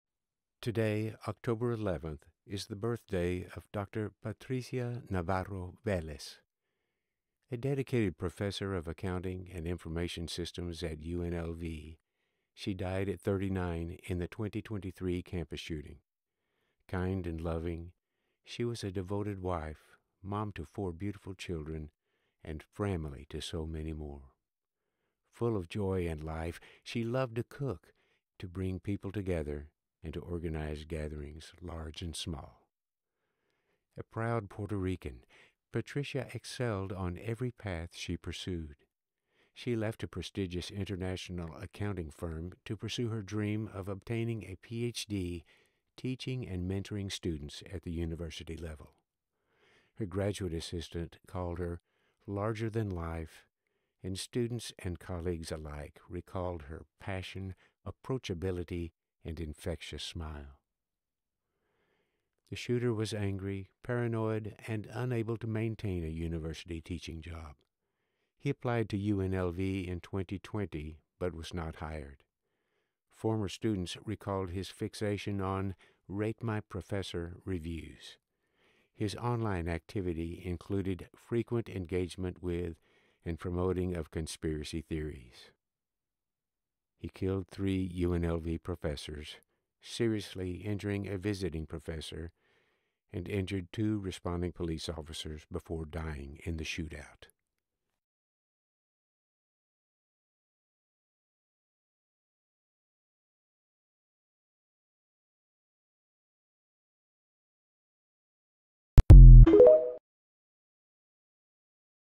Oct 11 Patricia Velez
Today, October 11th, is the birthday of Dr. Patricia Navarro Velez. A dedicated professor of accounting and information systems at UNLV, she died at 39 in the 2023 campus shooting. Kind and loving, she was a devoted wife, mom to 4 beautiful children, and “framily” to so many more. Full of joy and life, she loved to cook, to bring people together, and to organize gatherings, large and small.

A proud Puerto Rican, Patricia excelled on every path she pursued. She left a prestigious international accounting firm to pursue her dream of obtaining a PhD teaching and mentoring students at the university level. Her graduate assistant called her “larger than life,” and students and colleagues alike recalled her passion, approachability, and infectious smile.

The shooter was angry, paranoid, and unable to maintain a university teaching job. He applied to UNLV in 2020 but was not hired. Former students recalled his fixation on “Rate My Professor” reviews. His online activity included frequent engagement with and promoting of conspiracy theories. He killed 3 UNLV professors, seriously injured a visiting professor and injured 2 responding police officers before dying in the shootout.